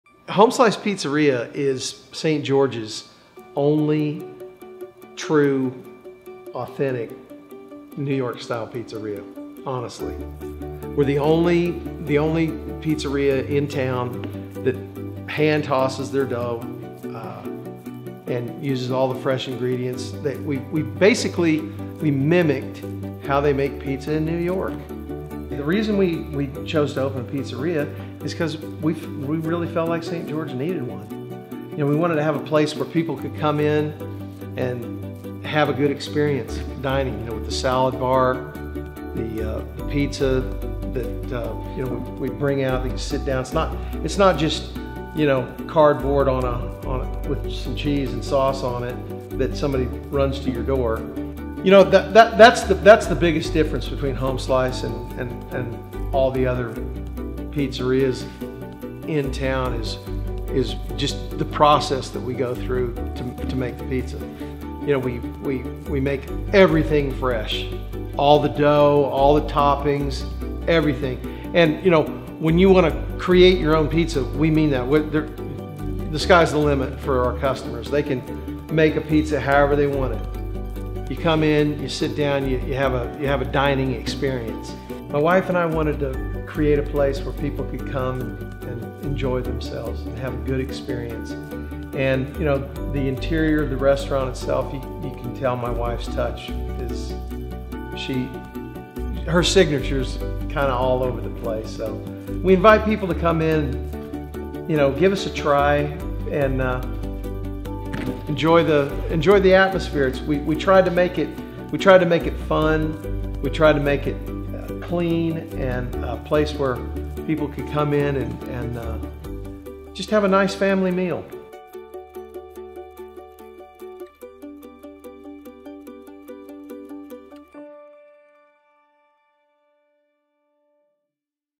Home Slice Pizzeria Short
A great local brick oven pizzeria with awesome family values!